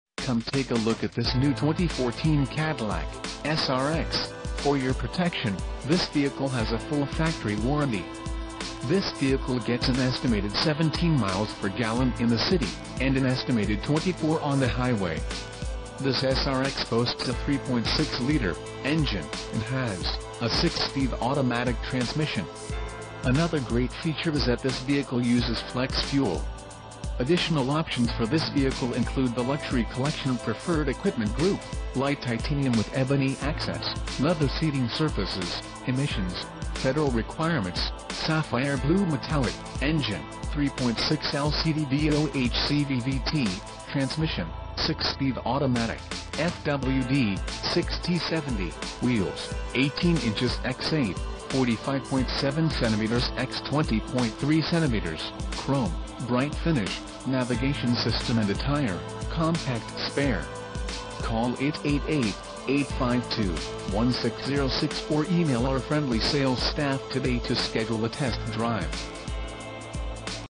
2014 Cadillac SRX FWD 4dr Luxury Collection SUV - Columbia, SC
Jim Hudson Buick GMC Cadillac, Columbia, SC - Leather, Nav, UltraView Sunroof, Chrome 18s, Bose Surround Sound, Power Liftgate